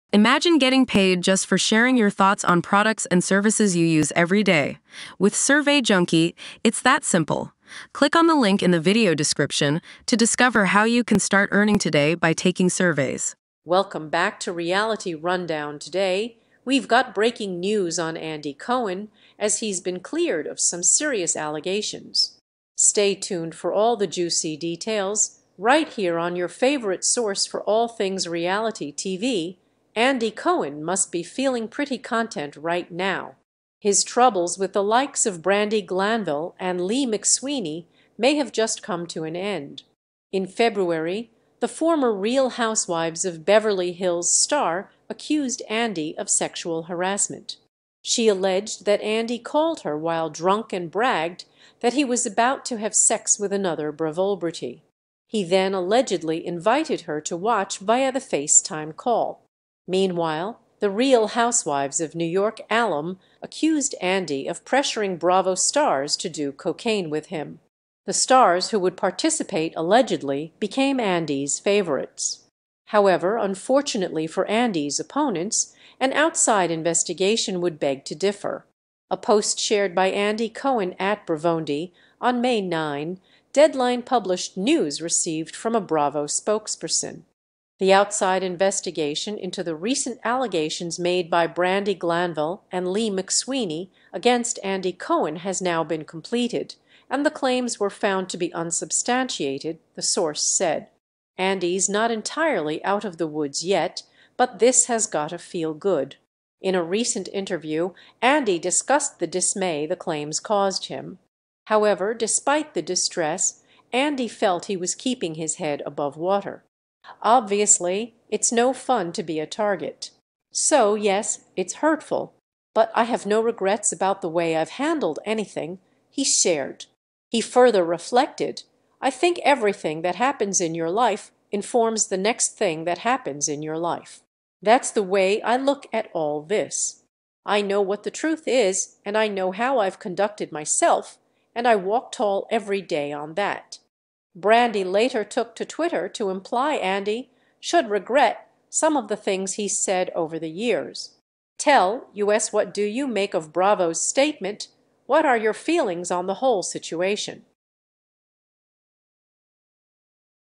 Breaking News: Andy Cohen Cleared of Brandi Glanville and Leah McSweeney's Unsubstantiated
In this breaking news update, we bring you the latest on Andy Cohen being cleared of the unsubstantiated allegations made by Brandi Glanville and Leah McSweeney. Tune in to find out the details and the outcome of this investigation. Stay informed with us!